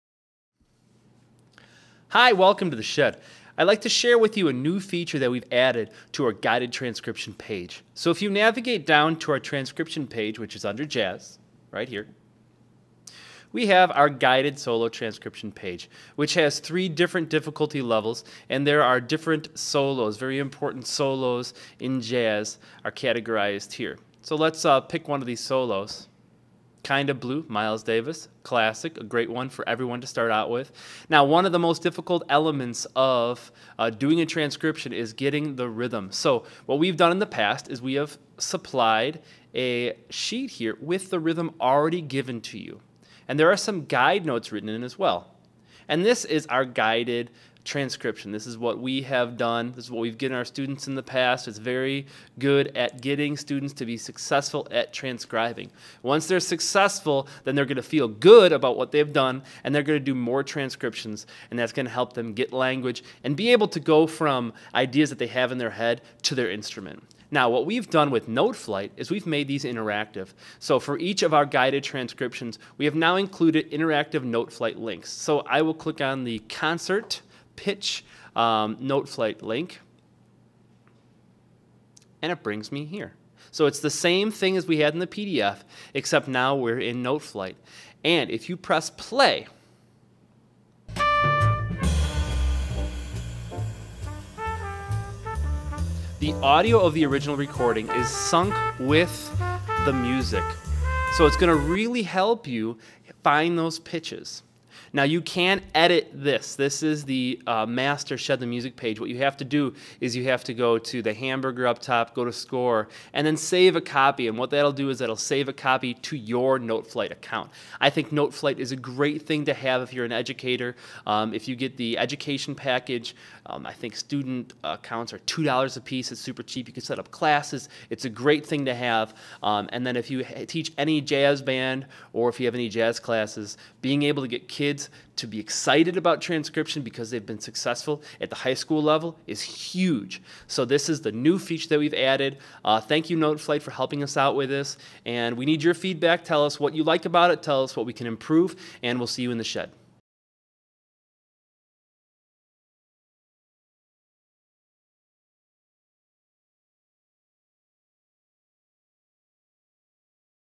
Guided transcription using Noteflight
We have added Noteflight scores to our guided transcriptions.